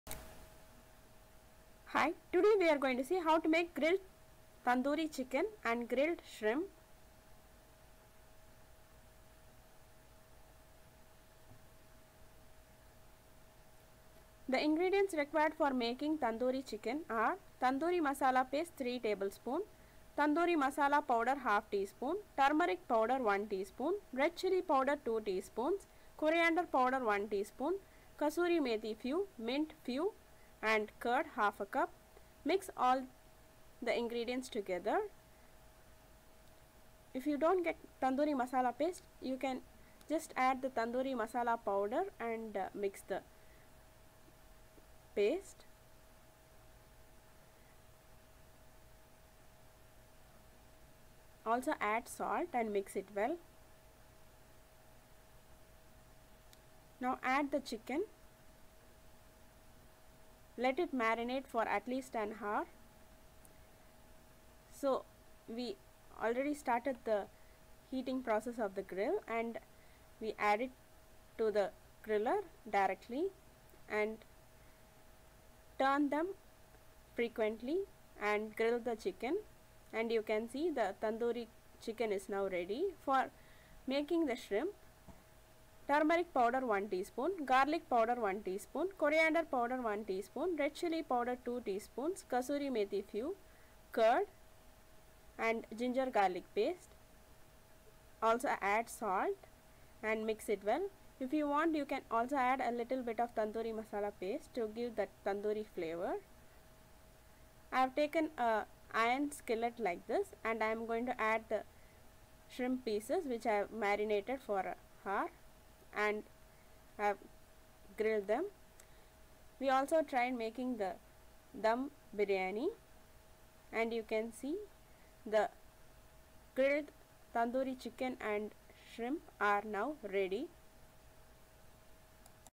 Grilled Tandoori Chicken and Grilled Shrimp Recipe
Ingredients :

Chicken - boneless pieces
For Making Tandoori Chicken :
1. Tandoori Masala Paste - 3 Tbs
2. Tandoori Masala Powder - 1/2 Ts
3. Turmeric Powder - 1 Ts
4. Red Chili Powder - 2 Ts
5. Coriander Powder - 1 Ts
6. Kasoori Methi - few
7. Mint - few
8. Curd / Yogurt - 1/2 cup
and Salt

Also can add some lemon juice and 1 Ts of Oil

For Making Grilled Shrimp :

Shrimp
1. Turmeric Powder - 1 Ts
2. Garlic Powder - 1 Ts
3. Coriander Powder - 1 Ts
4. Red Chili Powder - 2 Ts
5. Kasoori Methi - few
6. Curd / Yogurt - 1/2 cup
7. Ginger Garlic Paste and Salt

Mix all the masala ingredients given under for making tandoori chicken, mix well, add chicken pieces and marinate for at least one hour. Now grill them by turning them on both sides to cook it well, the recipe is ready

Mix all the ingredients given under for making grilled shrimp, mix well, add shrimp and marinate for at least one hour. Then take an iron skillet and place the shrimp on it and place the skillet directly on the grill and grill it, the recipe is ready